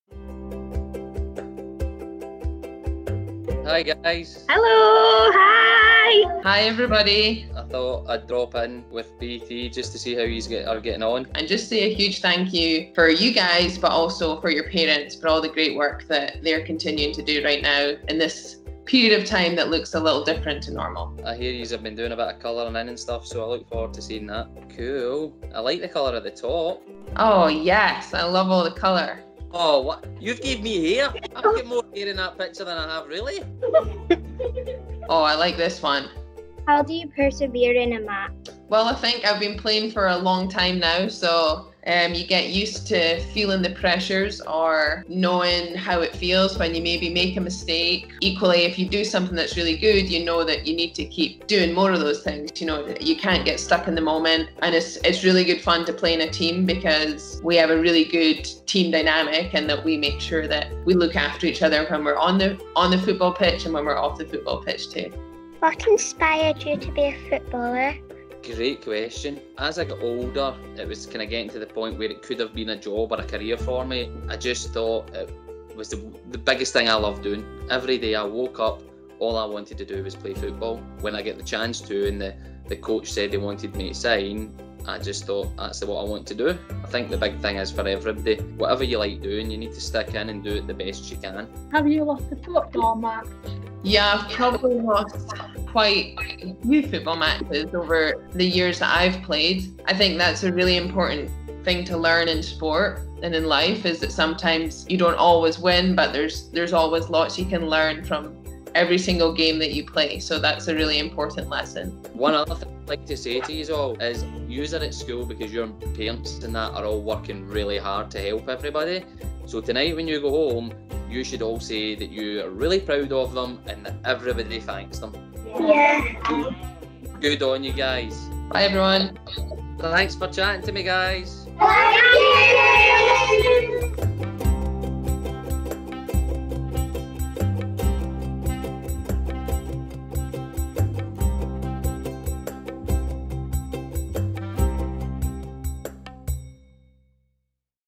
Rachel Corsie & Steven Naismith Surprise Young Fans | BT Key Connections
We teamed up with our lead partner BT to surprise Juniper Green Primary School and Hillside Primary School with visits from Rachel Corsie and Steven Naismith.

They spoke to the children of key workers to lift their spirits and thank their parents for the work they're doing right now.